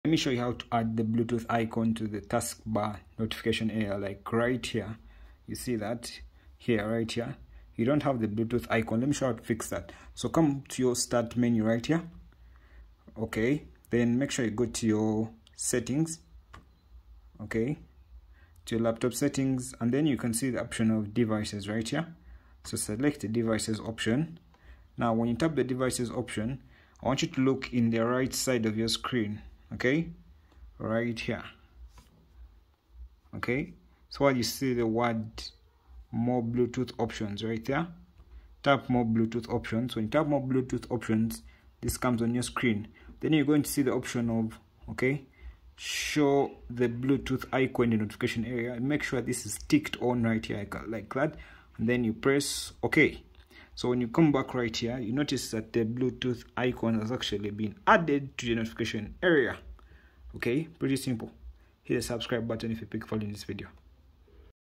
How TO Add Bluetooth Icon to task bar notification area laptop
let me show you how to add Bluetooth icon to the Taskbar notification area in your computer